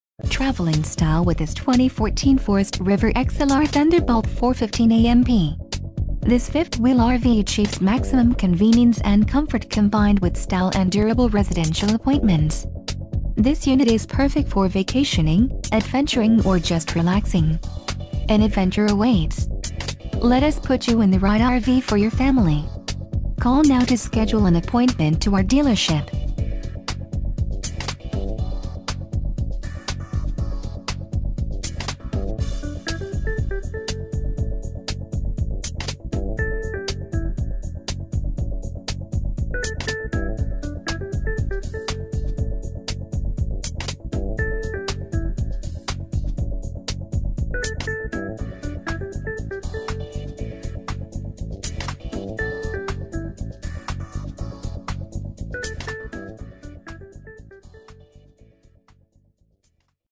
2014 Forest River XLR Thunderbolt - Tom Raper - Richmond, I
You\'ll be inspired for greatness in this victory red 42\'1\" fifth wheel by Forest River! This 2014 XLR Thunderbolt 415AMP toy hauler model is decked out in a bourbon interior decor with wide triple slide outs and room for up to 10 guests comfortably! View this fabulous option today at our Richmond RV sales location! , 2nd Air Conditioner\r\n5.5kw Onan Carb Evap Generator\r\n2nd Elecctric Awning\r\nFull Body Paint W/premium Gel Coat Sidewalls\r\nMobile Fuel Station W/fuel Tank\r\nElectric Bed W/bottom Pass Thru Dinette\r\nFall Promotion Pkg\r\nVip Party Deck W/awning\r\nBourbon Decor\r\nVictory Red Full Body Paint